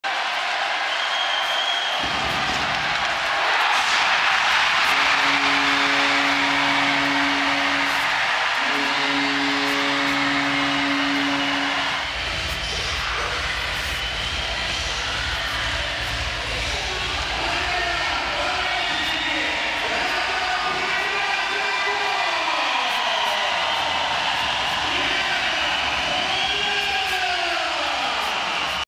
ГОЛ Филипп Майе 2 Металлург Мг v Спартак 2-1 КХЛ 2022-23#shorts
ГОЛ. Филипп Майе 2. Металлург Мг v Спартак. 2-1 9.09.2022